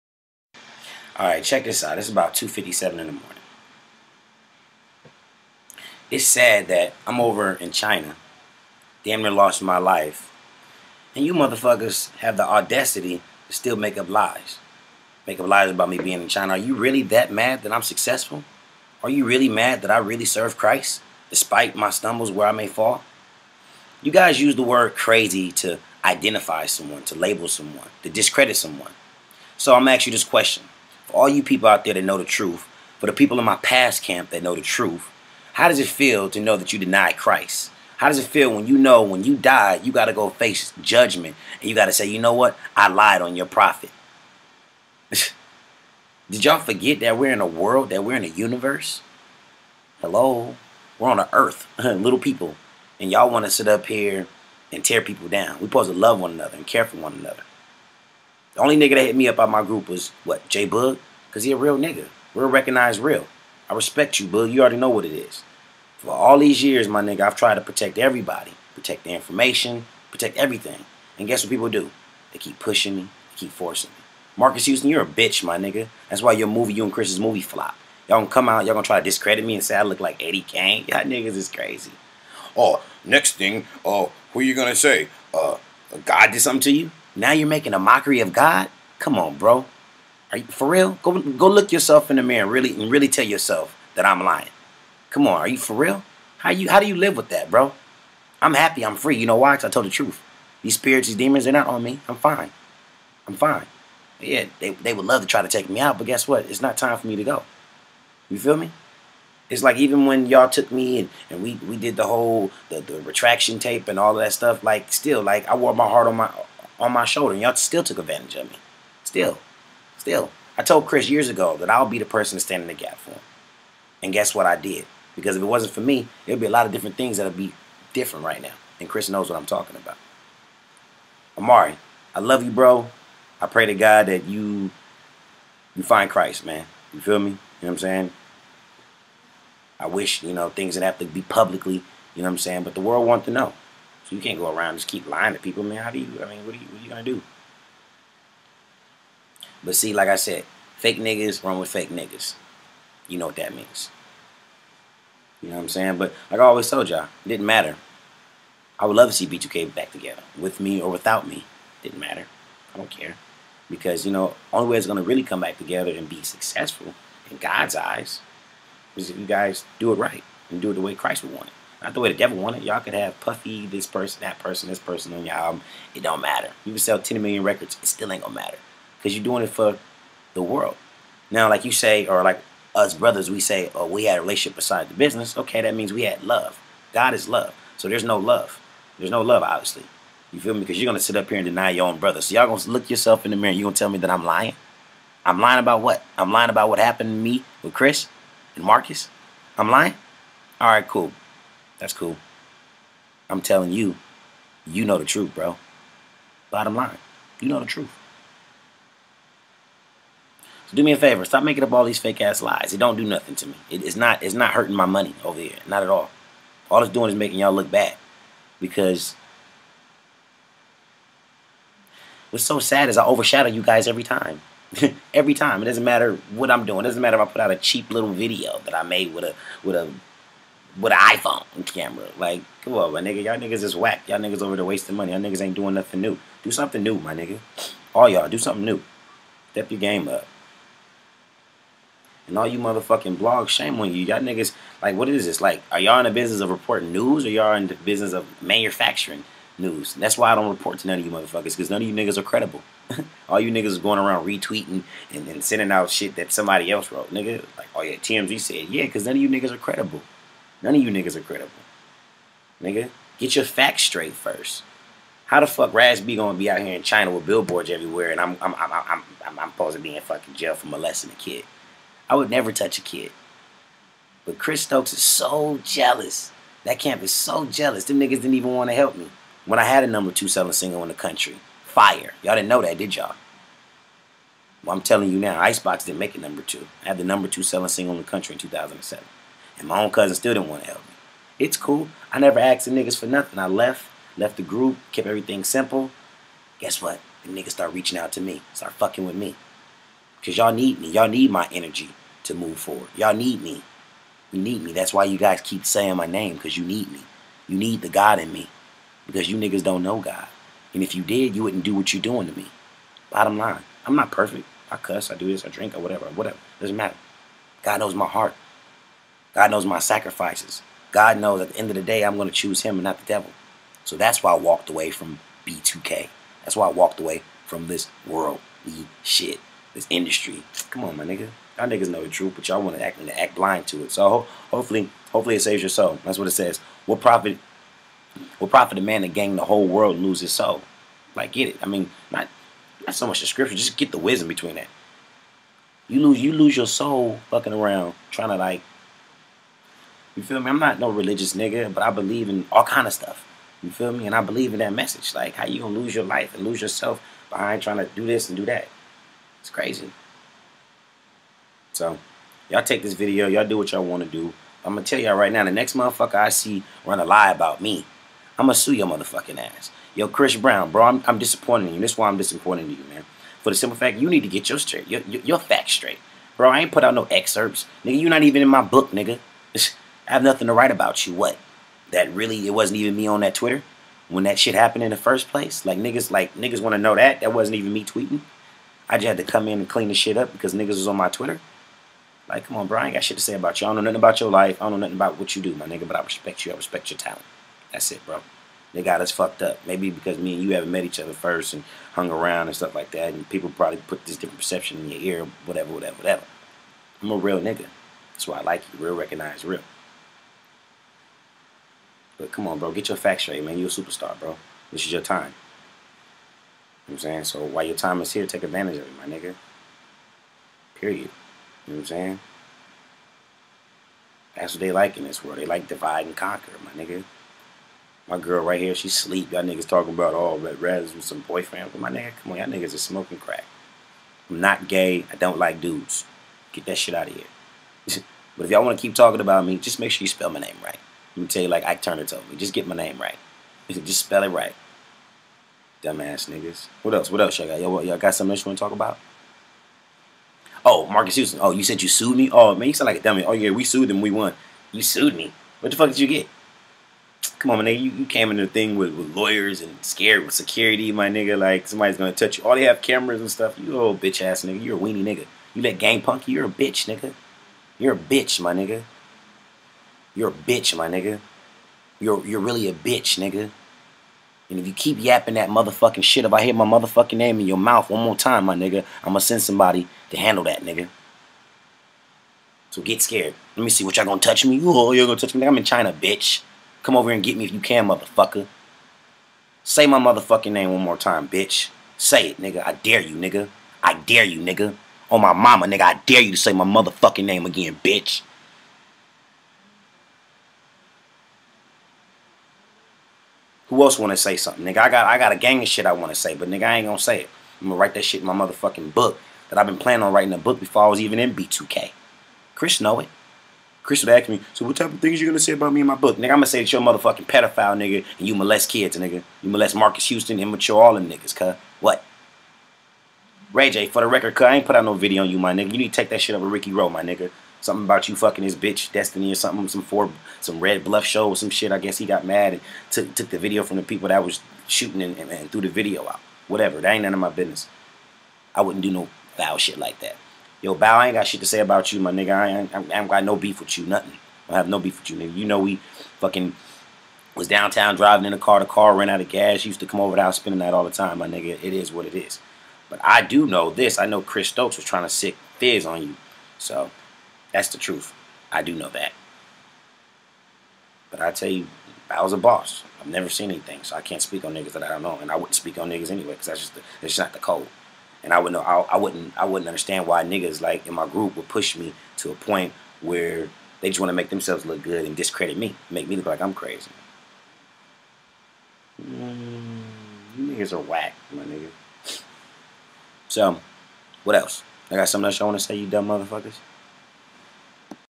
Ima Send Somebody To Handle That: Raz B Airing It Out After Nearly Losing His Life!
Raz B Addressing Again...